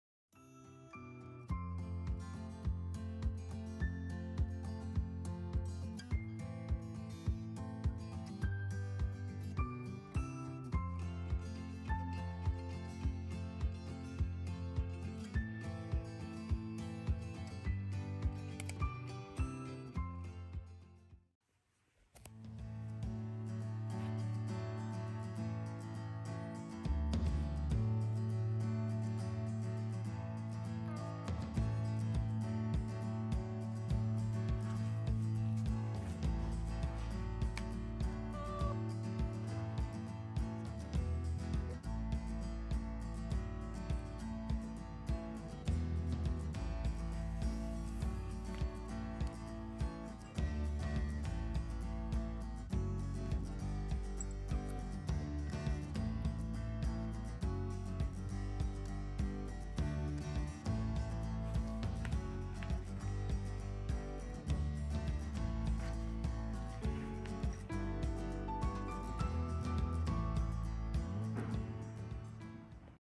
猫もオフトゥン好きなんだね【テラス】
人間も猫もオフトゥンにはかなわないらしい( *´艸｀)